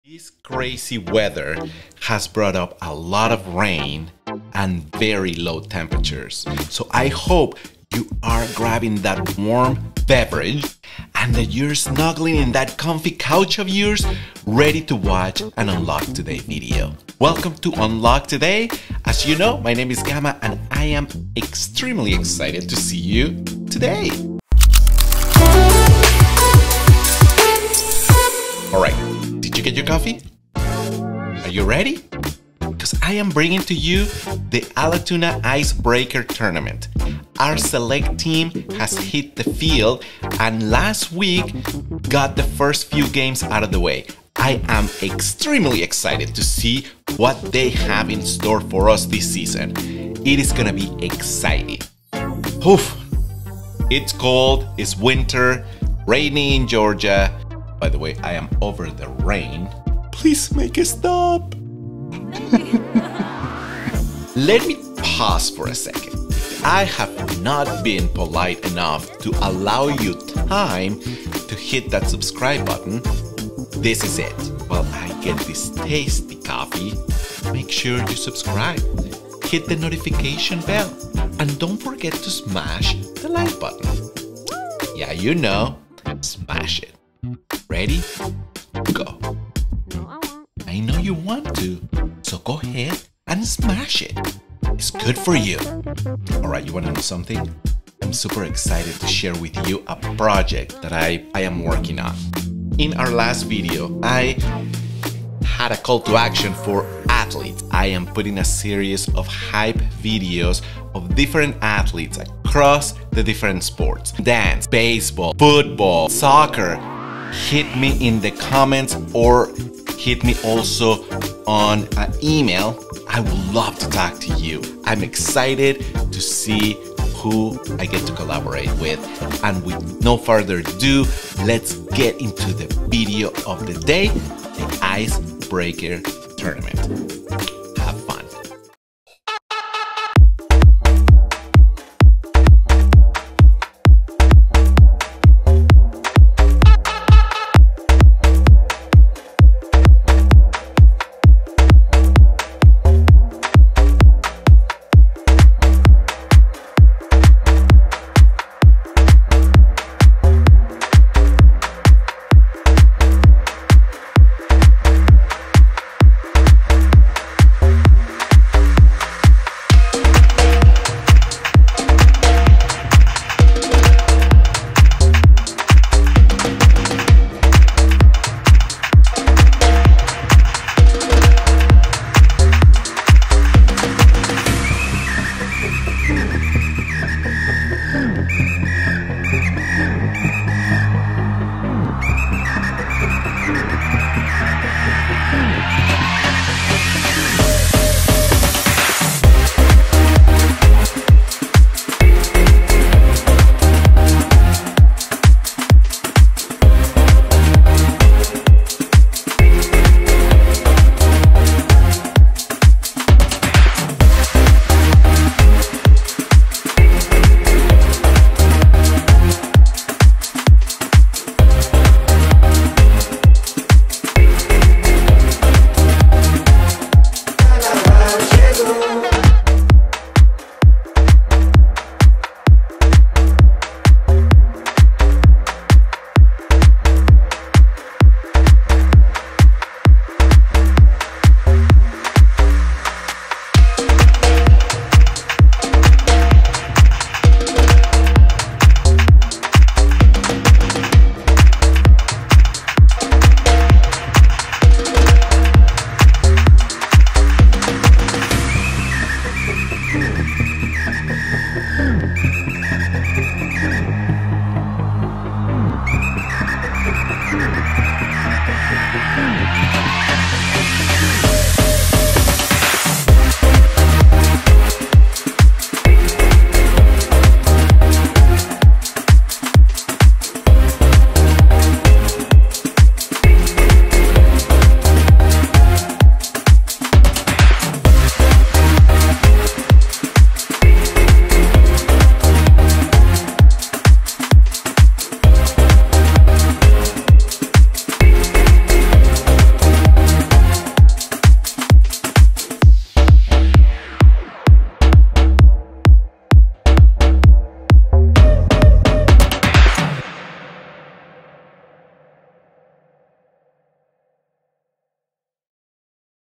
ICEBREAKER | LAX TOURNAMENT
A new LAX season is about to start and the Wolfpack select team makes its first appearance on a cold day.
Get ready for some heart-pumping, nail-biting action as the Wolfpack select team takes the field in the ICEBREAKER tournament! Watch as they battle it out against the toughest opponents and showcase their skills on the field. Let's cheer on the Wolfpack and show them some love!